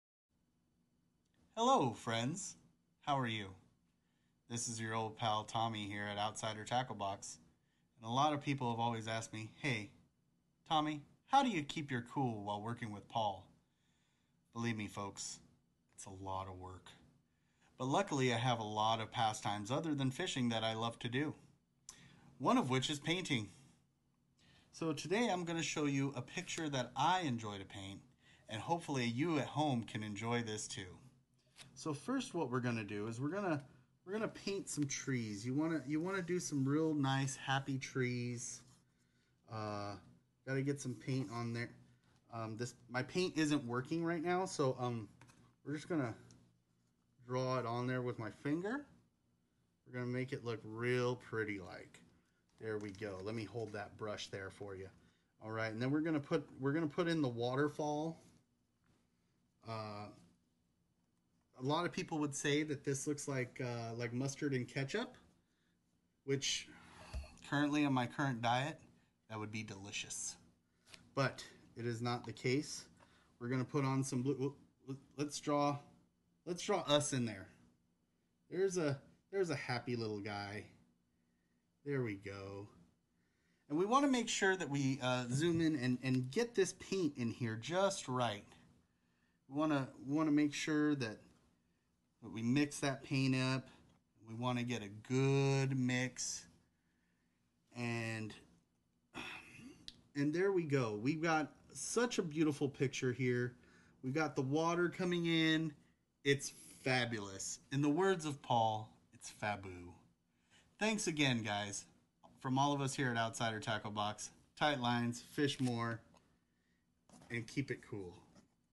Painting with Tommy
How Tommy Relaxes for the Podcast. Painting Nature for fisherman is relaxing. Tommy creates a master piece using left over card board from OTB box.